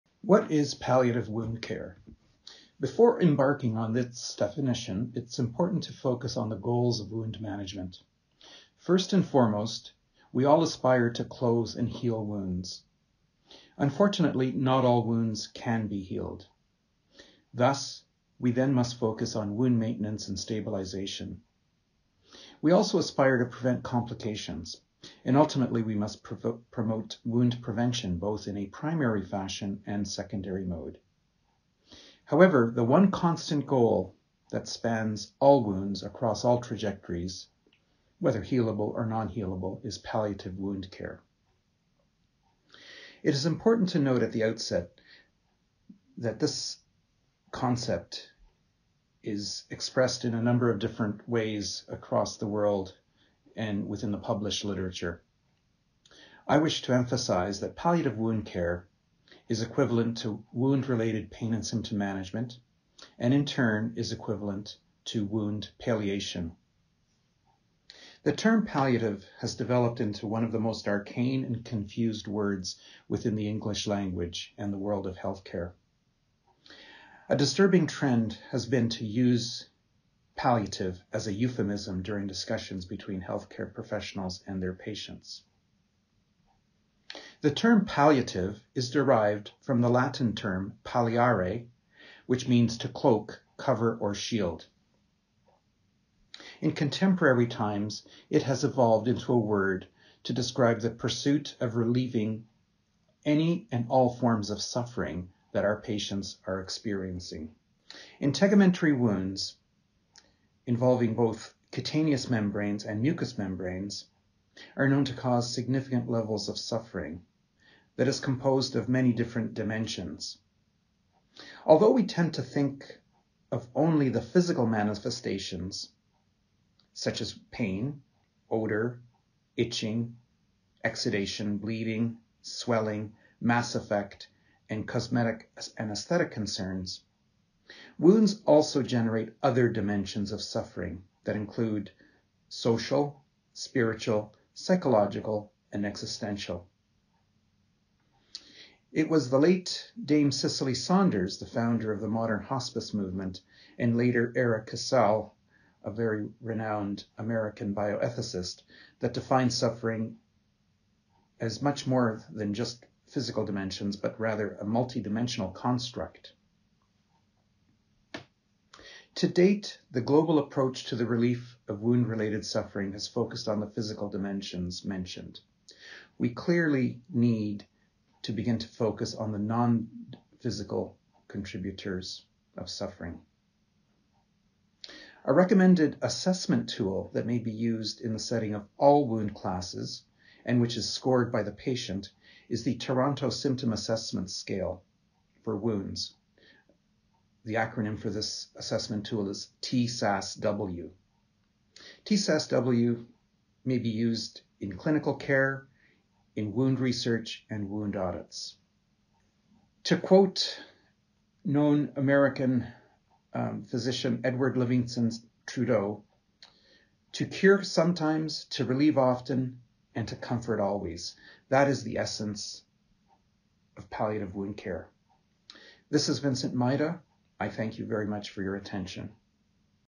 Podcast Episode 32: Palliative wound care
Dr Vincent Maida MD, MSc, BSc, CCFP(PC), FCFP, ABHPM, Associate Professor University of Toronto, Canada and President & CEO, VinSan Therapeutics Inc.  Listen to Dr Maida discussing what is palliative wound care.